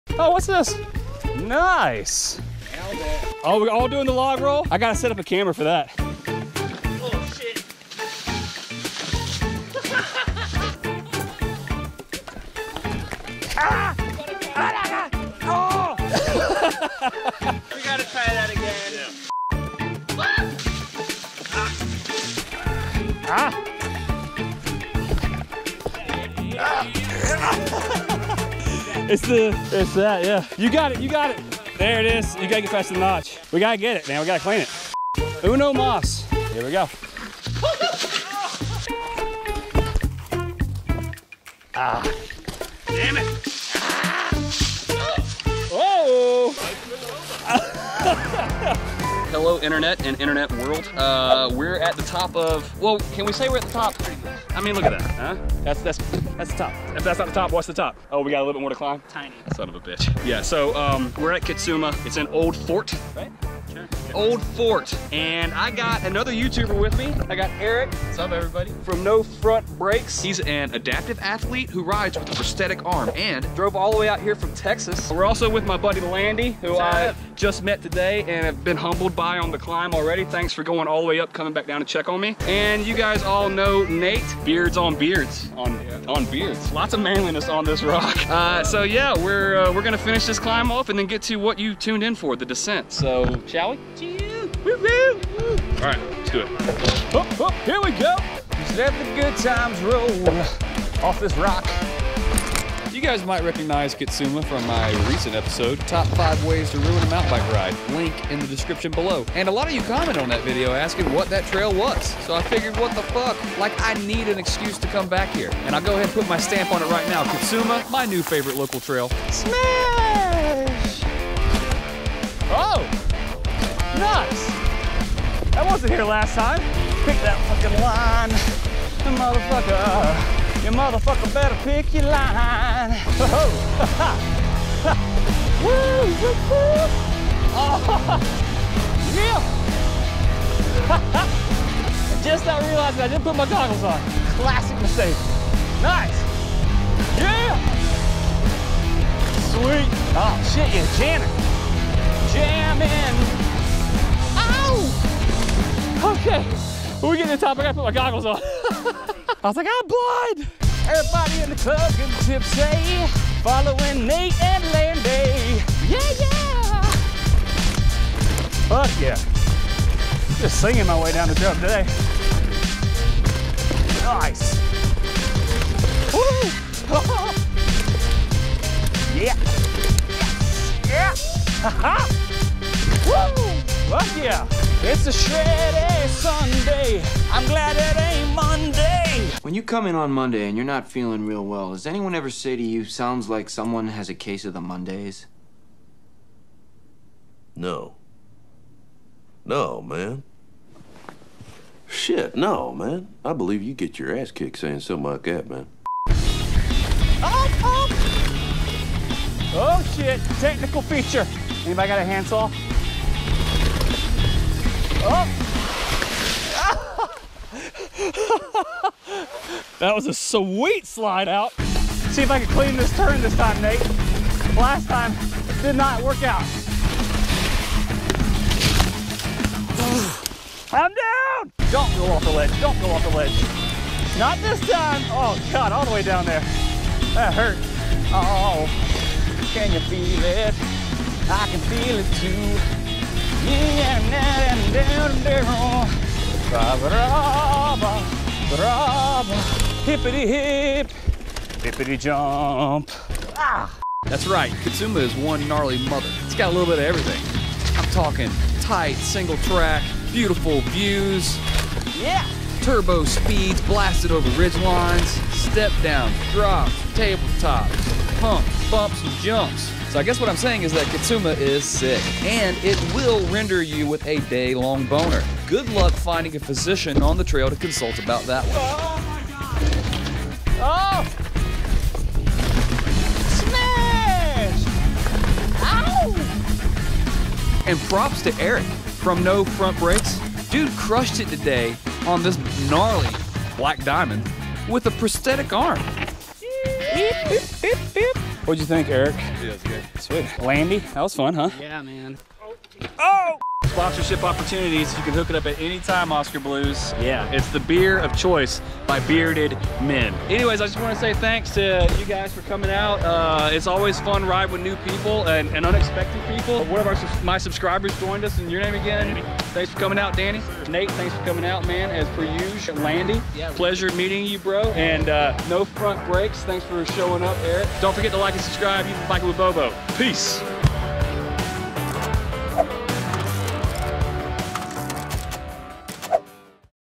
Beards, Beers and No Front Brakes?
My beardliest buddies and I , hit up my new favorite local trail "Kitsuma".